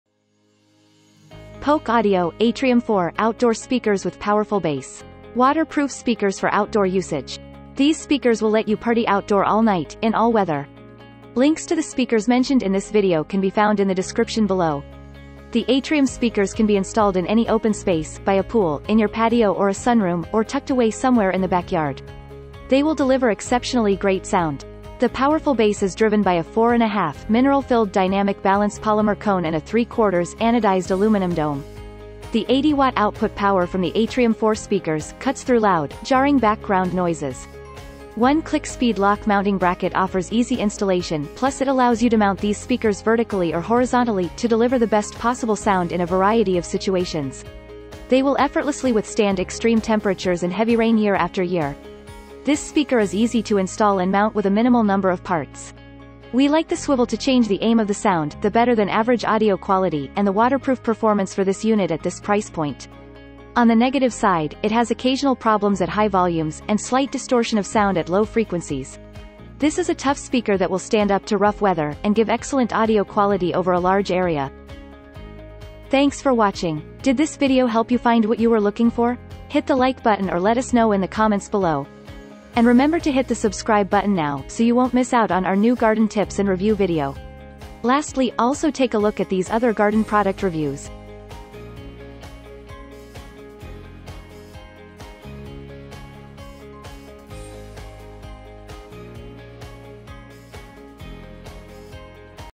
🌻 Polk Audio, Atrium 4 Outdoor Speakers with Powerful Bass Review
Great Water Proof Speakers For Outdoor Usage:
🌿Polk Audio Atrium 4 Outdoor Speakers

We have analyzed 5538 customer reviews and looked at 1 product to give you this top 1 video review.

Features:
#1 AM4088-A, Polk Audio Atrium 4 Outdoor Speakers with Powerful Bass
 - Great Sound
 - Powerful Bass
 - 80W Output
 - Vertically/Horizontally Installation
 - This speaker is easy to install and mount with a minimal number of parts. We like the swivel to change the aim of the sound, the better than average audio quality, and the waterproof performance for this unit at this price point. On the negative side, it has occasional problems at high volumes and slight distortion of sound at low frequencies. This is a tough speaker that will stand up to rough weather, and give excellent audio quality over a large area.

Garden Life is a participant in the Amazon Services LLC
provide a means for sites to earn advertising fees by advertising